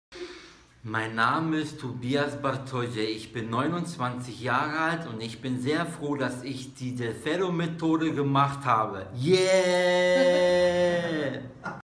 Stottern muss nicht sein: Del Ferro
Tobias ist sehr froh das er die Del Ferro Methode gemacht hat...